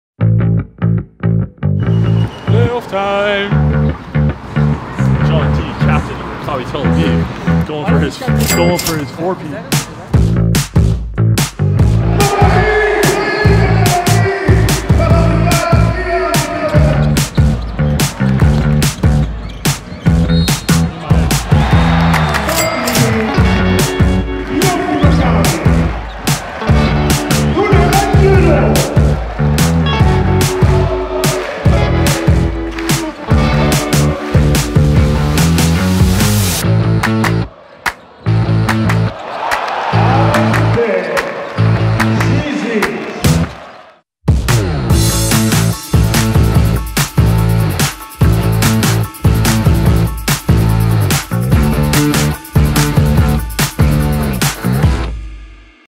Playoff time in Tel Aviv
מכבי Playtika תל אביב פתחה את הפלייאוף של עונת 2020/21 עם ניצחון 56:67 על הפועל באר שבע